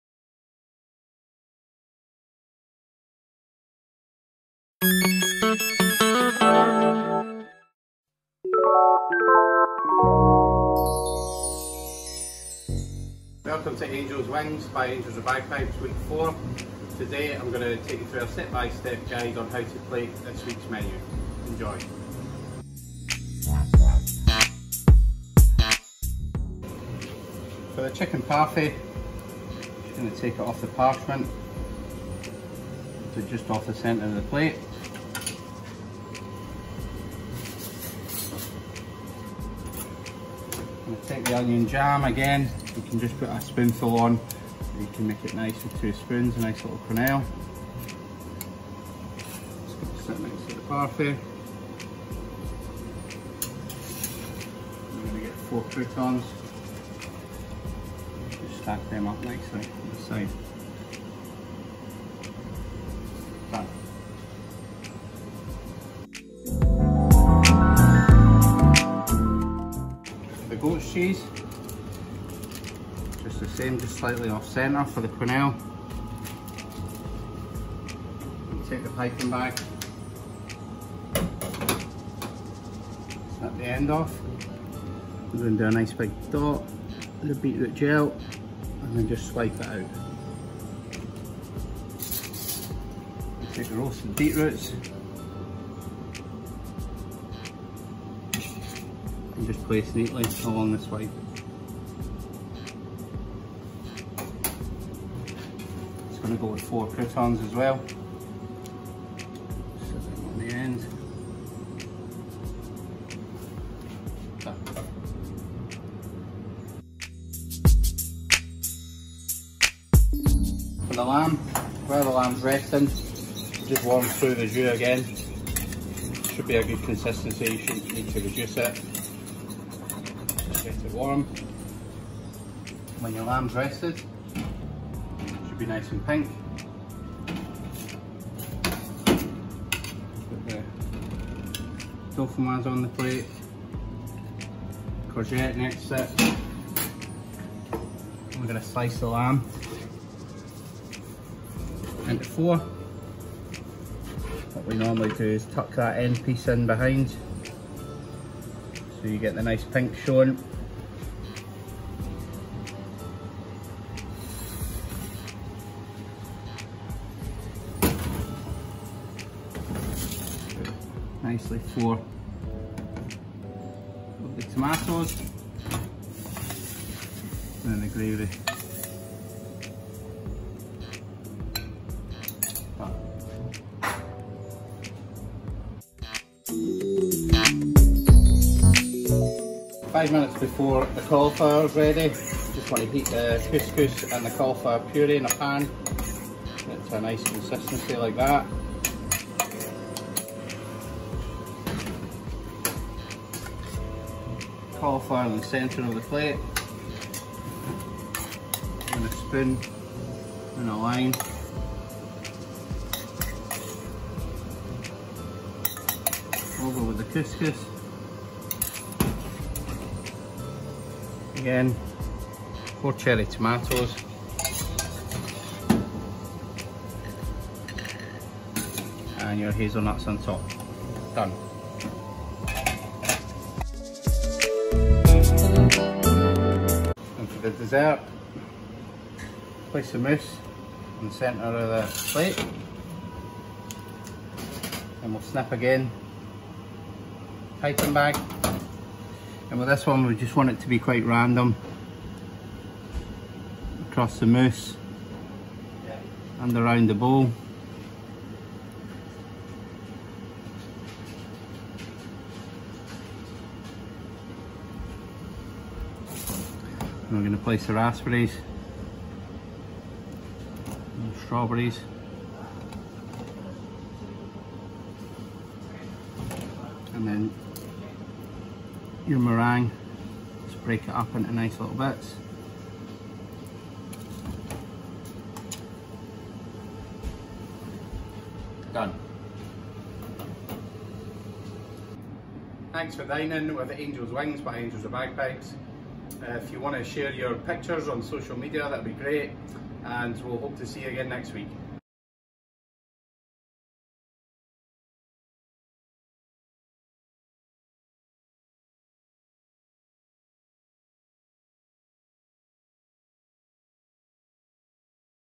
Angels Wings Week Four
Facebook: Angels With Bagpipes Restaurant & Bar

Please note, correct PPE is worn for proper prep... Fraser only prepared this without PPE so you could understand him!